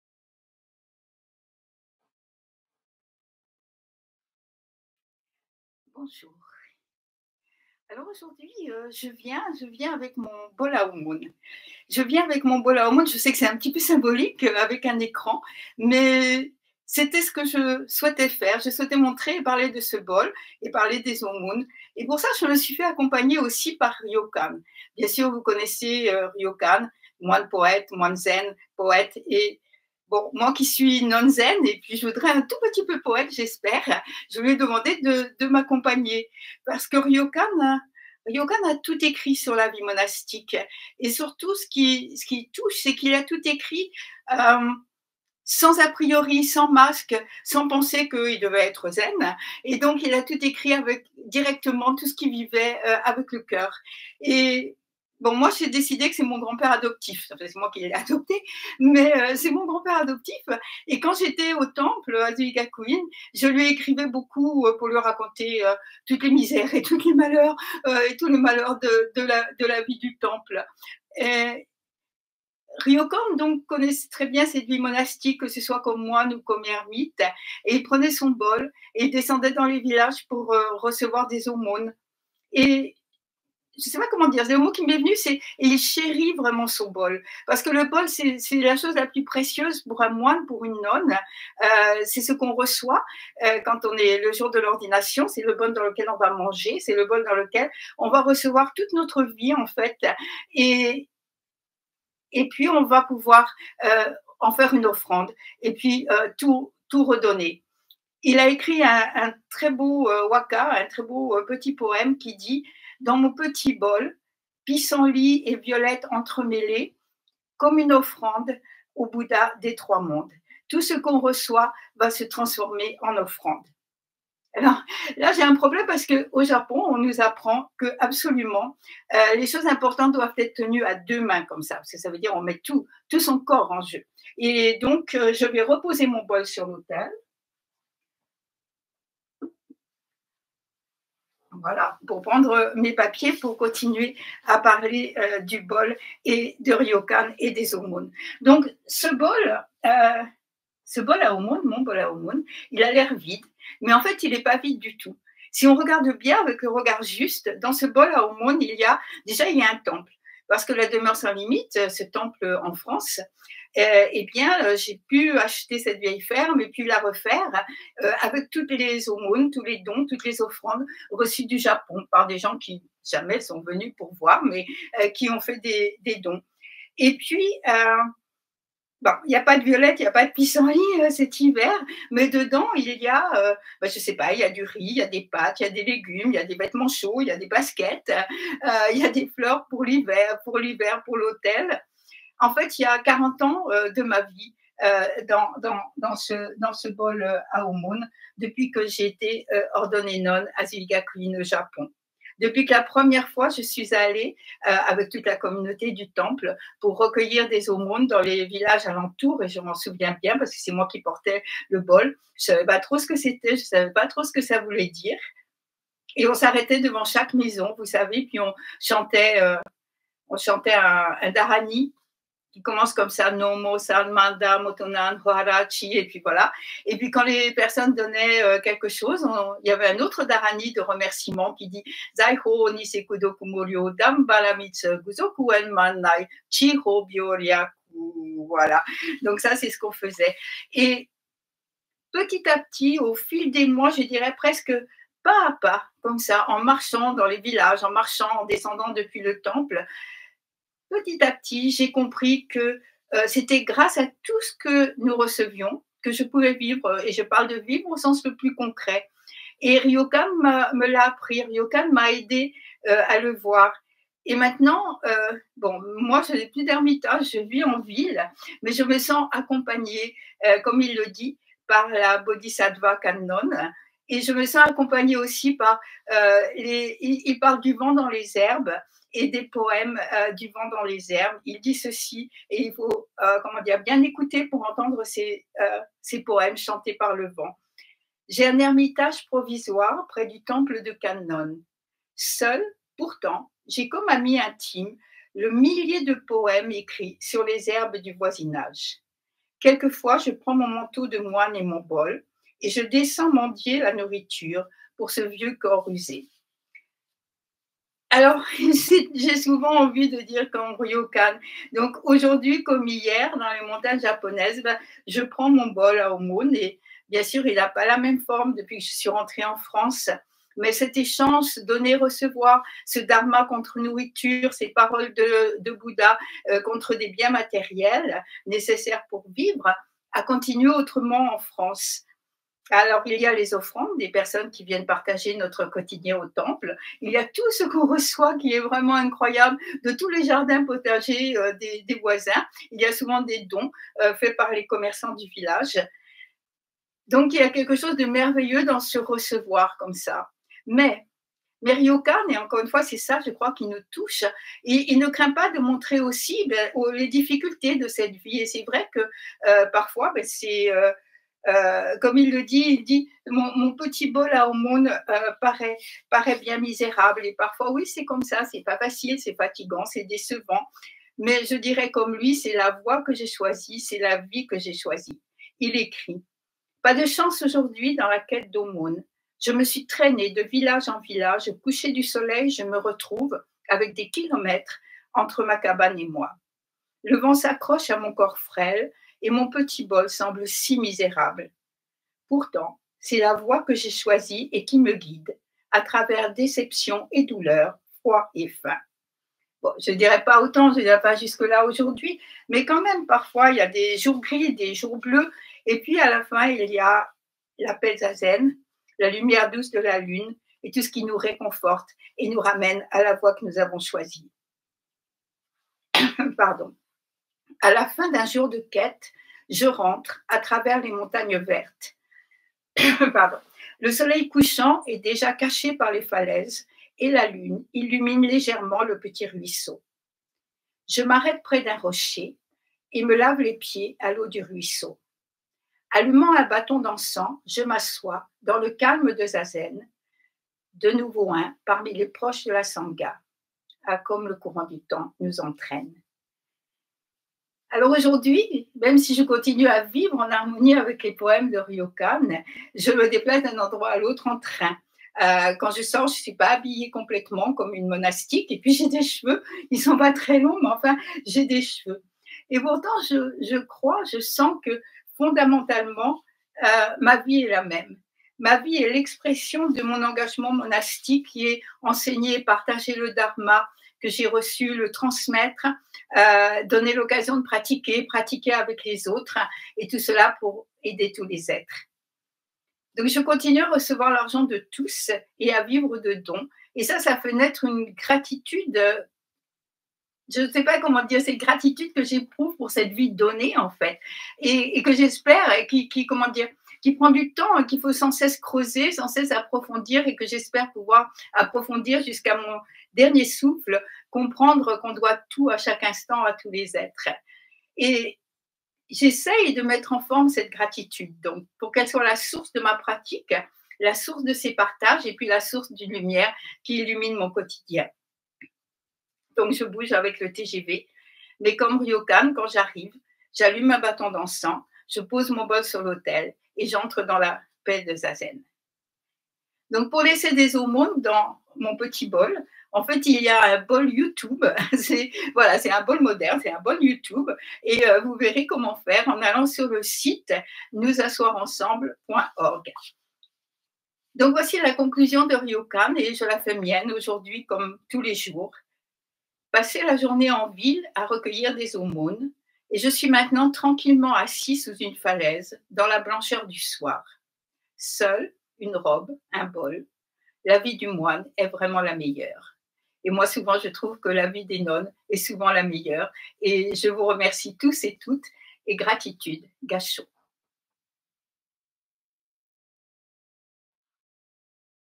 Violettes et pissenlits...
Version courte, partage seulement.
Aujourd’hui, j’ai pris mon bol à aumônes… c’est un peu symbolique avec un écran, c’est vrai !  Et j’ai décidé de me faire accompagnée par Ryokan, moine-poète du Zen, moi qui suis nonne et, j’essaie un peu, poète du Zen.
Ryokan a tout écrit  de cette vie de moine, sans masque, sans a priori, sans l’idée qu’il devrait « être zen ! »
C’est mon grand-père adoptif, je lui ai souvent écrit pour lui dire les joies et les difficultés de ma vie au temple du Japon...
Ryokan vit d’aumônes, il descend dans les villages alentour, et il chérit son petit bol, il sait que vivre d’aumônes c’est recevoir sa vie
à chaque instant de l’univers entier, et redonner tout ce que l’on a reçu, en faire à son tour une offrande...Donner-recevoir sans fin.

« Dans mon petit bol à aumônes,
des violettes et des pissenlits
tout entremêlés -
comme une offrande
aux Bouddhas des Trois Mondes. »

Et moi, je présente aujourd’hui mon bol à aumônes ; il a l’air vide mais il n’est pas du tout vide : pas de  violettes ni de pissenlits cet hiver, mais  un temple, puisque la DsL a été établie à partir de toutes les aumônes reçues au Japon, et il y a du riz et des légumes et des vêtements chauds, et  oui des fleurs pour l’autel, et puis il y a toute ma vie, 40 ans de ma vie, depuis que j’ai été ordonnée nonne bouddhiste au japon à Zuigakuin.

Depuis que pour la première fois, je sus allée au village avec toute la Communauté, et je portai le bol, et je ne savais pas trop ce que ça voulait dire...(…)

Donc aujourd’hui sur YouTube comme hier dans les villages des montagnes japonaises, je prends mon bol à aumônes. Bien sûr, il n’a pas la même forme depuis que je suis revenue en France.
Mais cet échange, ce donner-recevoir, Dharma et nourriture, Paroles du Bouddha et biens matériels nécessaires pour vivre, a continué autrement: offrandes des personnes qui viennent partager notre quotidien, dons venus directement des potagers des voisins, des commerçants du village aussi..(…)

Je me déplace d’un endroit à l’autre en train ; à l’extérieur, je ne m’habille pas complètement en monastique, j’ai des cheveux, courts mais des cheveux !
Et pourtant je sens, je crois que fondamentalement ma vie est la même, ma vie est l’expression de mon engagement monastique: enseigner, partager le Dharma que j’ai reçu, le transmettre, donner l’occasion de pratiquer, et pratiquer ensemble, pour le bien de tous les êtres..

Je continue à recevoir l’argent de tous, à vivre de dons ; ça faire naître la gratitude ...j’essaye de mettre en forme cette gratitude, donc, qu’elle soit la source de ma pratique, la source de ces partages, la source qui illumine mon quotidien..

Pour laisser des aumônes dans mon bol, il y a un « bol- YouTube », vous verrez comment faire en  allant sur le site de nousasseoirensemble.org.

Voici la conclusion de Ryokan, et je la fais mienne :

« Passé la journée en ville
à recueillir des aumônes,
et je suis maintenant tranquillement

  assis sous une falaise
   dans la blancheur du soir.
   Seul – une robe et un bol!
La vie du moine est vraiment la meilleure. »

La vie de la nonne aussi ! Merci !

Gratitude. Gassho